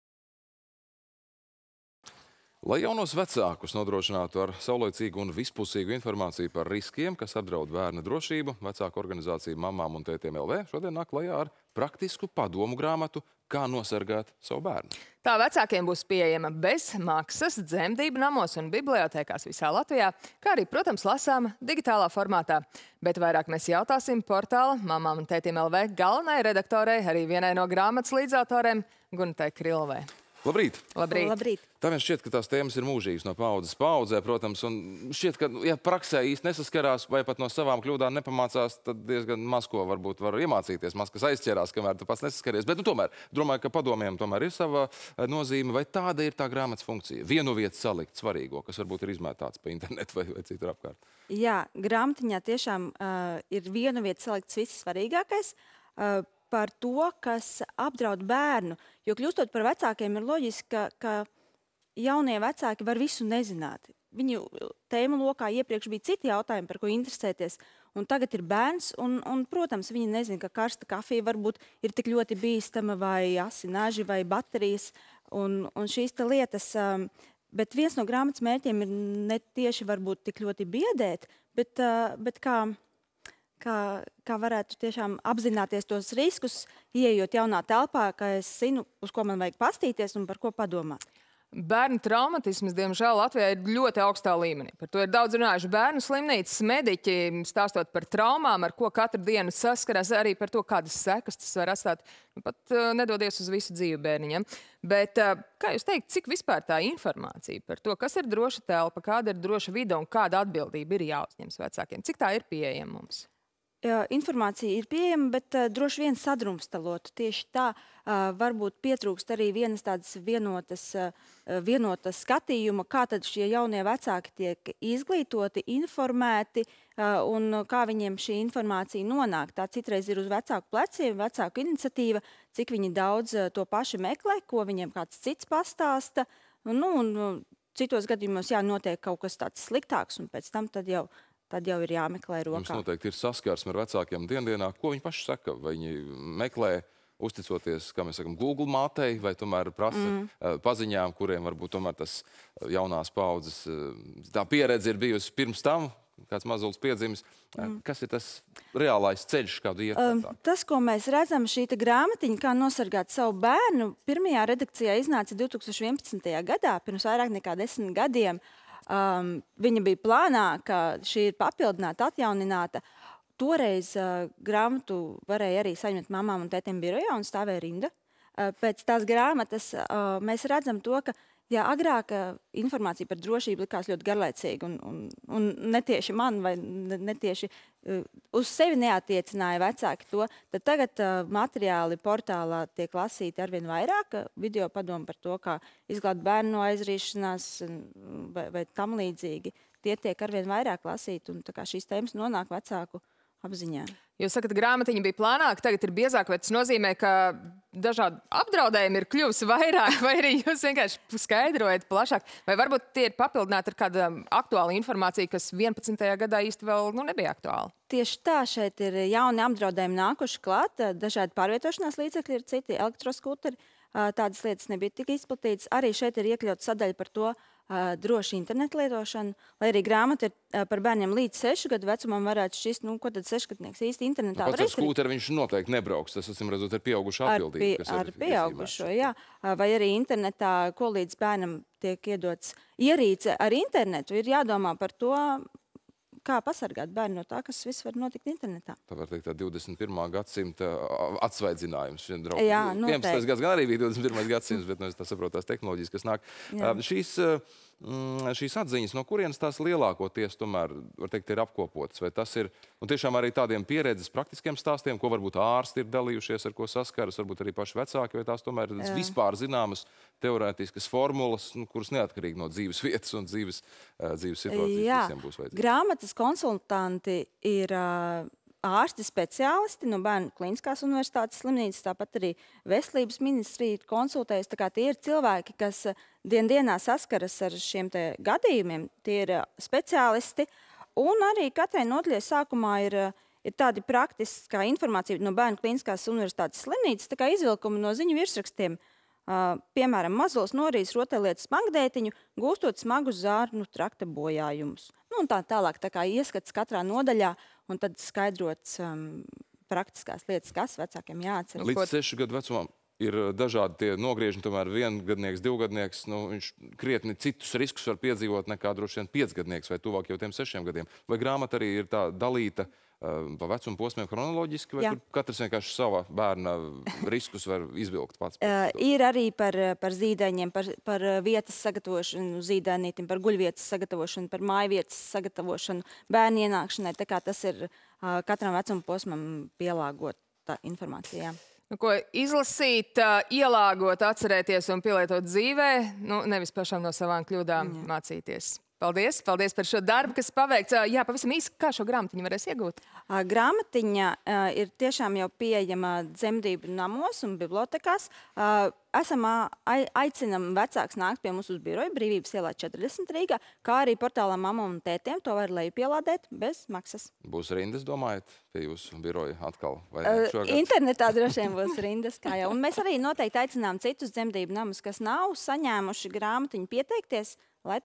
Intervija ar ģimenes portāla Mammamuntetiem.lv galveno redaktori Gunitu Krilovu
Lai jaunos vecākus nodrošinātu ar savlaicīgu un vispusīgu informāciju par riskiem, kas apdraud bērna drošību, vecāku organizācija “Mammamuntetiem.lv” šodien nāk klajā ar praktisku padomu grāmatu “Kā nosargāt savu bērnu”.
Tā vecākiem būs pieejama bez maksas dzemdību namos un bibliotēkās visā Latvijā, kā arī lasāma digitālā formātā. Bet vairāk jautāsim portāla Mammamuntetiem.lv galvenajai redaktorei, arī vienai no grāmatas līdzautorēm Gunitai Krilovai.